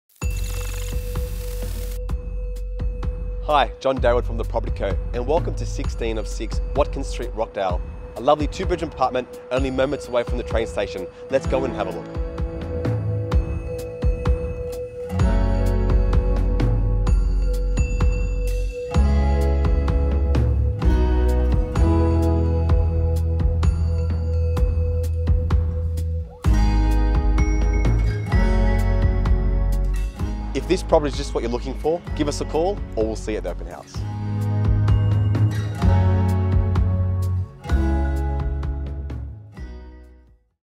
FOR LEASE - 16/6 Watkin St Rockdale presented by The Property Co
John Daewoud from The Property Co Group presents this conveniently located apartment, minutes from the train station in Rockdale.